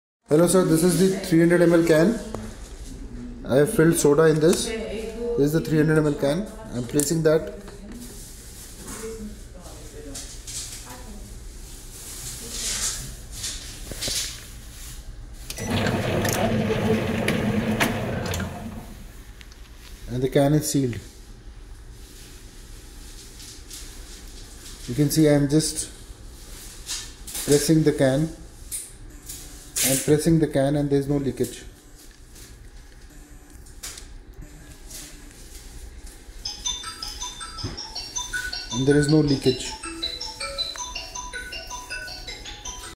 PET CAN FOR BEVERAGE AND CAN SEAMING MACHINE
We are manufacturers of pet cans for packing various beverages.

Sizes available from 100ml to 500 ml.

We make cans for both carbonated and non carbonated beverage.

We also provide can seaming Machine to seal the Pet can with the help of easy open ends.